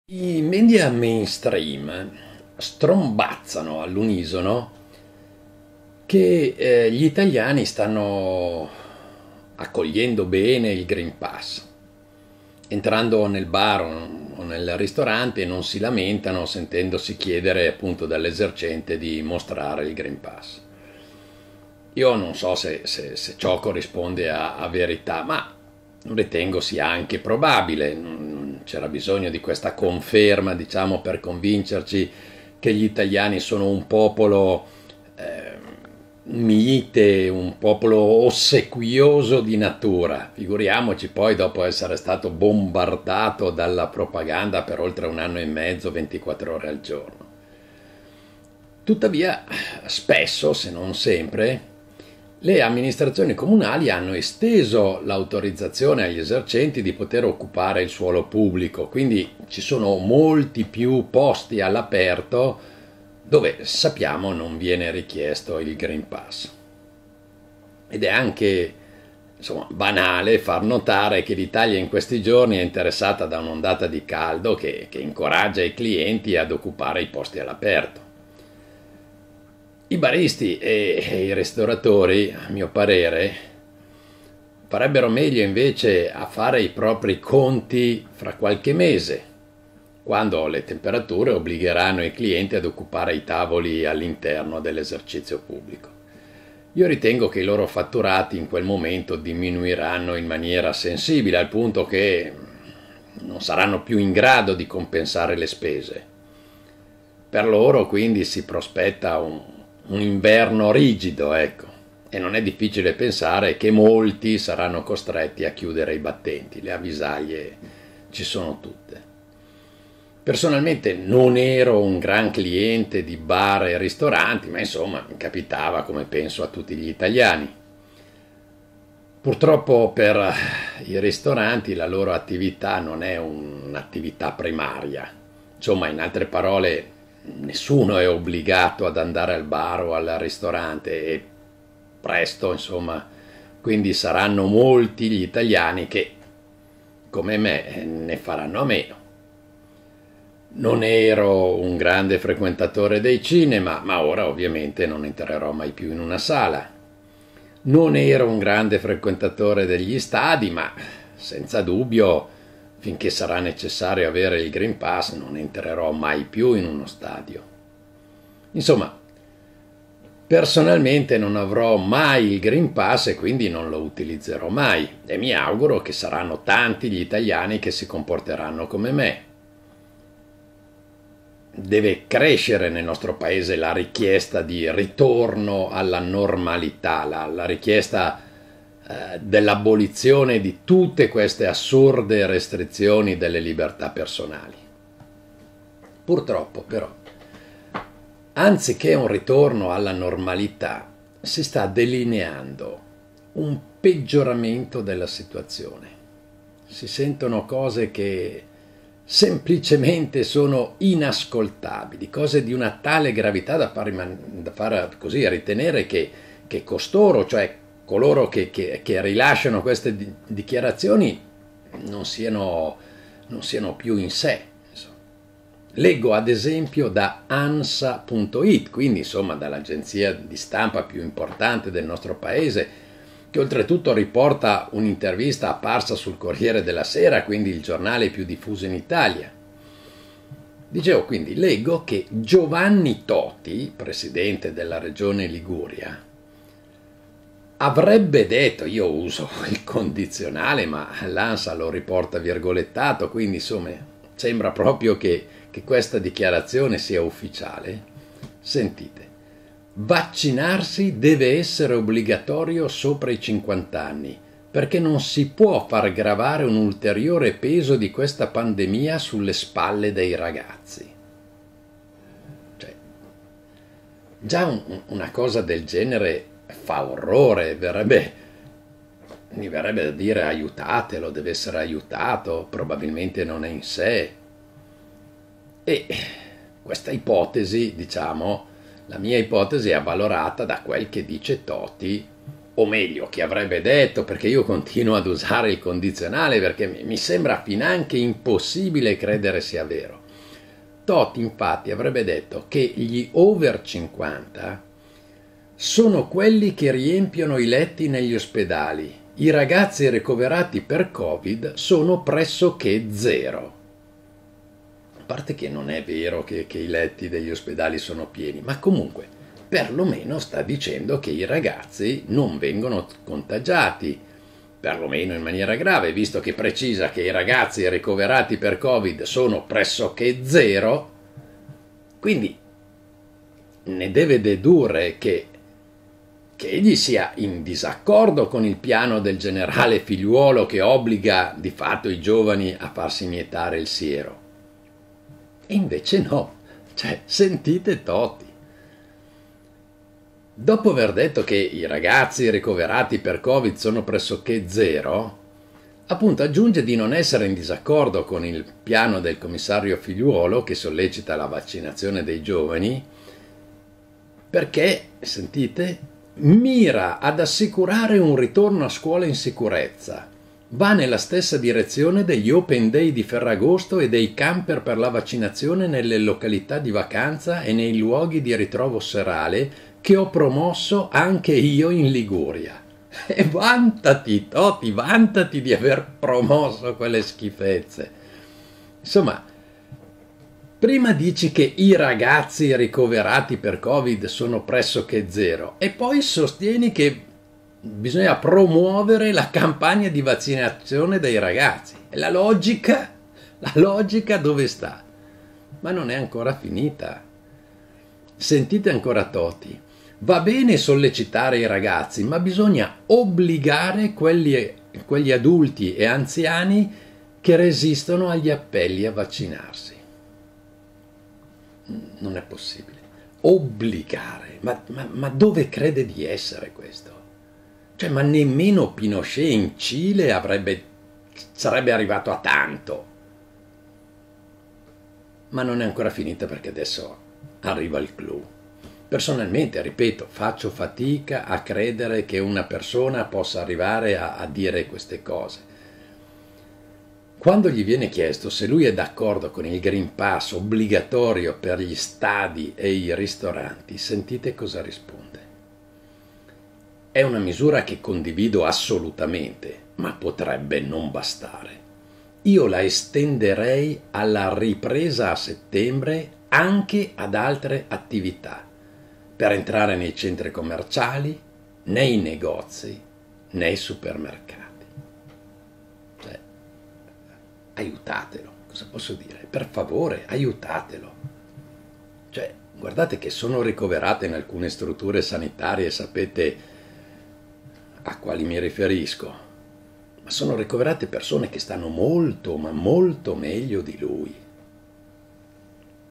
TOTI, NON QUELLO DELLA STAMPELLA
Pare che gli italiani abbiano accolto il green pass senza obiezioni, probabilmente la calura torrida di questi giorni invoglia i clienti a sedersi all'esterno dei locali. Quest'inverno le cose saranno diverse ed i ristoratori se ne accorgeranno.
Intanto Toti (non Enrico, l'eroe della stampella), ma Giovanni il Presidente della Liguria rilascia delle dichiarazioni deliranti.